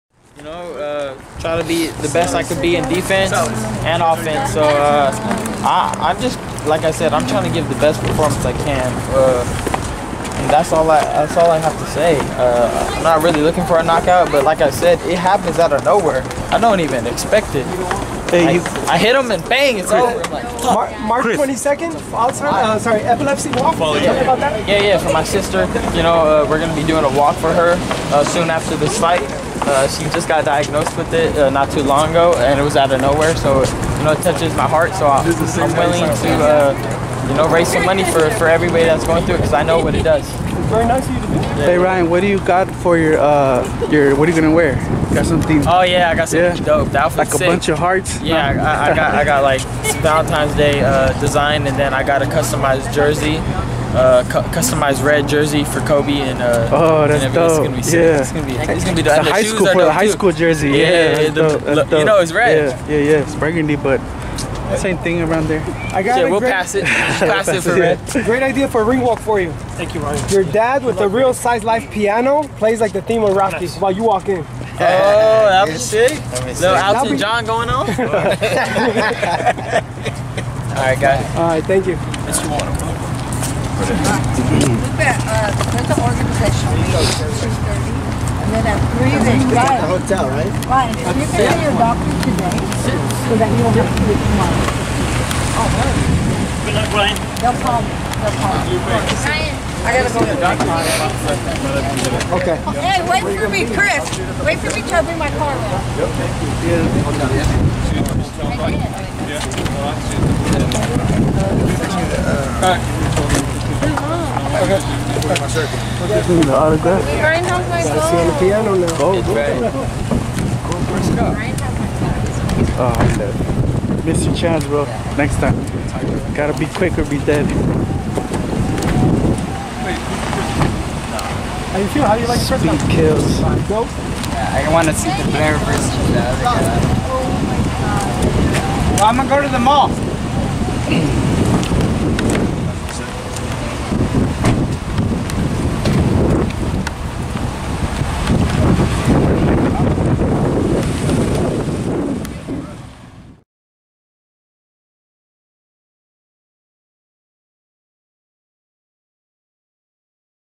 RYAN GARCIA REVEALS KOBE BRYANT HOMAGE & "SICK" OUTFIT PLANNED FOR VALENTINE'S DAY SHOWDOWN
FightHype.com was on hand in Anaheim, California where lightweight sensation Ryan Garcia held the final press conference ahead of his upcoming Valentine's Day showdown with contender Francisco Fonseca. You don't want to miss what those in attendance had to say. Check it out!